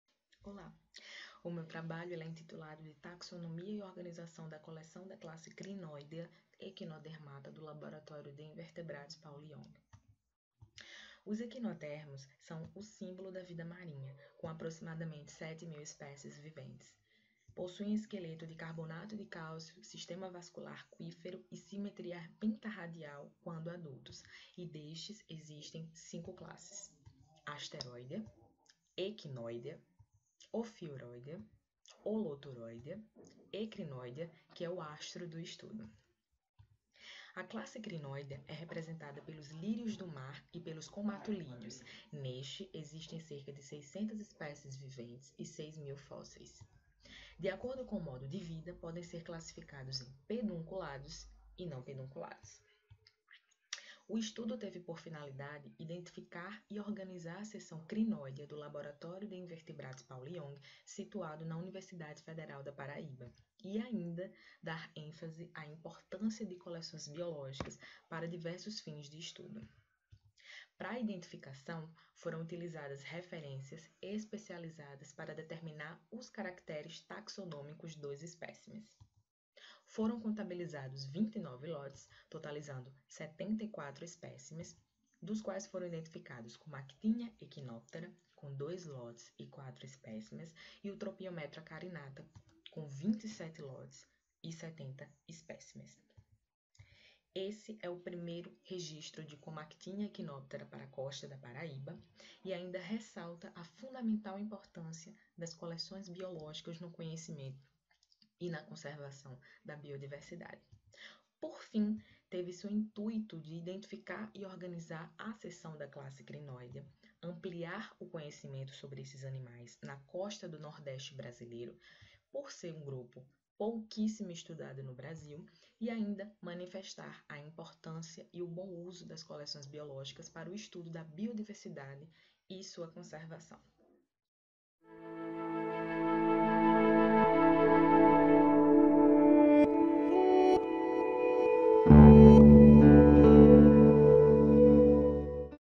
TAXONOMIA E ORGANIZAÇÃO DA COLEÇÃO DA CLASSE CRINOIDEA (ECHINODERMATA) DO LABORATÓRIO (..)
APRESENTAÇÃO DE TRABALHOS - SÁBADO (31/10)

Título do Trabalho: TAXONOMIA E ORGANIZAÇÃO DA COLEÇÃO DA CLASSE CRINOIDEA  (ECHINODERMATA) DO LABORATÓRIO DE INVERTEBRADOS PAULO YOUNG

Autores: CONCEIÇÃO, T. E.

Resumo: Os equinodermos (gr. echinos, espinho; ouriço + derma, pele) são o símbolo da vida marinha, com  aproximadamente 7.000 espécies viventes. Eles são brevemente definidos por possuir esqueleto  de carbonato de cálcio na forma de calcita; um sistema vascular aquífero que auxilia na  locomoção, captura de alimento, entre outras funções; e apresentar simetria pentarradial nos  adultos. A classe Crinoidea é representada pelos lírios do mar e comatulídeos, totalizando cerca de  650 espécies viventes e 6.000 espécies fósseis. Eles são subdivididos em pedunculados e não pedunculados. Aqueles que estão presos ao substrato por um pedúnculo, são comumente  chamados de lírios do mar, enquanto as formas não-pedunculadas são chamadas estrelas de pena  ou comatulídeos, compreendendo a ordem Comatulida. O objetivo deste estudo é identificar as  espécies e organizar a sessão de Crinoidea da Coleção de Invertebrados Paulo Young da  Universidade Federal da Paraíba. Para tanto, foram consideradas as referências especializadas  para determinar os caracteres taxonômicos, bem como seguido as metodologias apropriadas para  a identificação das espécies. Foram analisados 29 lotes, com 74 espécimes, e identificadas as  espécies Comactinia echinoptera (Família Comasteridae) e Tropiometra carinata (Família  Tropiometridae). Esse é o primeiro registro de Comactinia echinoptera para a costa da Paraíba. Na  costa brasileira, foram registradas 16 espécies pertencentes à classe Crinoidea. Em geral, a sua  baixa diversidade pode também estar associada à falta de especialistas desse grupo no Brasil, o  que é refletido na pequena quantidade de estudos sobre esses animais. As espécies citadas para a  costa do Nordeste, restringem-se à resultados de listas de espécies, sem discussão sobre sua  taxonomia e variações morfológicas. O estudo amplia o conhecimento para os crinoides do Brasil,  especialmente para a costa nordestina, com dados de distribuição e habitat das espécies.

OUVINTES QUE DESEJAREM FAZER PERGUNTAS, ACERCA DO TRABALHO, PODEM USAR A CAIXA DE COMENTÁRIOS, AQUI DO YOUTUBE, QUE DE 17 ÀS 18H, DO DIA DE HOJE, OS AUTORES ESTARÃO DISPONÍVEIS PARA RESPONDER AS QUESTÕES LEVANTADAS.